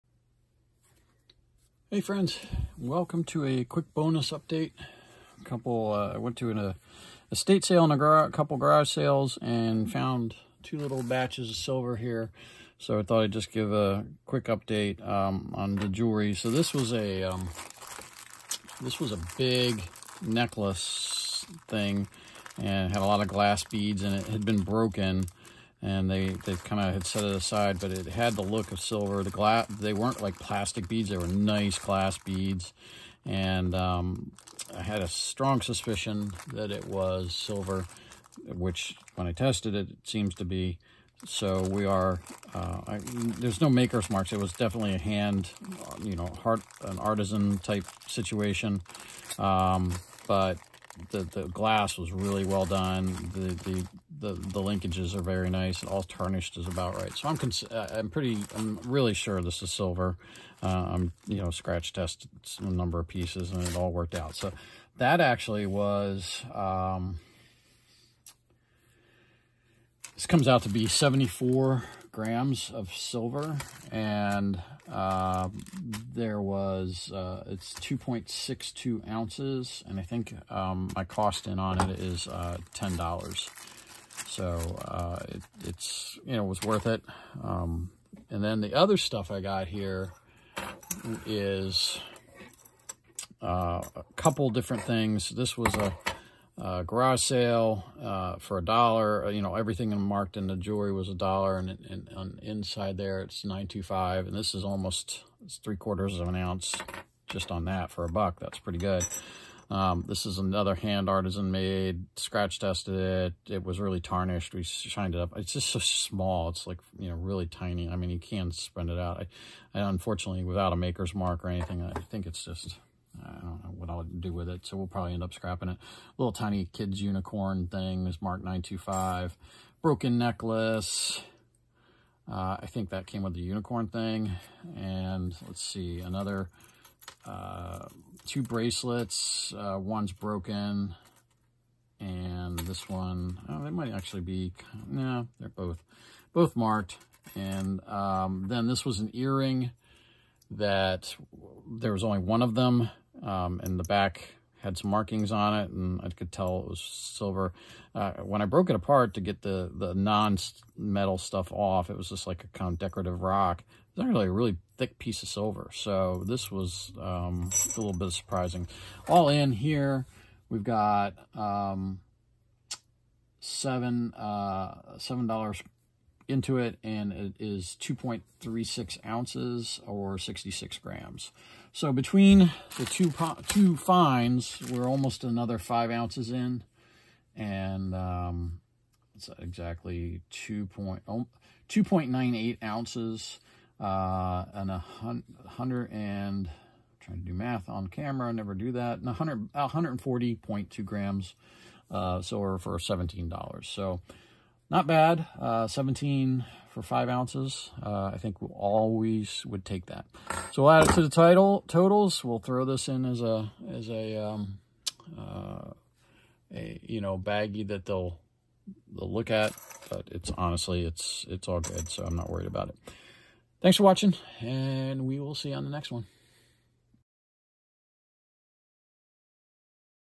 Scrap Silver into a Gold Coin Quest - Week 8 Bonus
How long will it take us to accumulate enough scrap Sterling silver to trade in for a 1-ounce gold coin?

Got super lucky and found an old piece of Sterling at the local thrift store.

Always buying & selling Role Playing Games like Dungeons & Dragons.